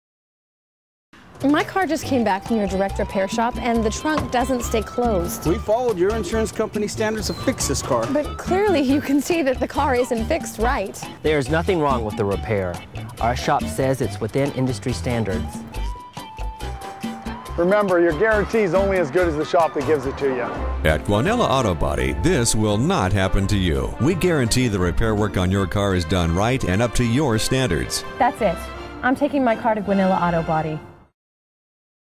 Your Standards - Guanella Auto Body
First and foremost, our obligation is to YOU, the customer, and your vehicle. Guanella Auto Body does not have direct repair relationships with any insurance company. We have made a conscious decision to remain independent.

If you're in an accident, you have two choices: First, you can call your insurance company and put all your trust and faith in a corporation. They will advise you to take your car to one of their direct repair facilities.

Your second option is to call an independent body shop like Guanella, and let us deal directly with your insurer. We will work closely with you and deal with your insurer directly to make sure your vehicle is fixed properly.

In deciding what to do, there are two questions to ask yourself:

    Do you trust your insurer to take care of you and your vehicle?

    Or, do you want a say in how your vehicle is repaired?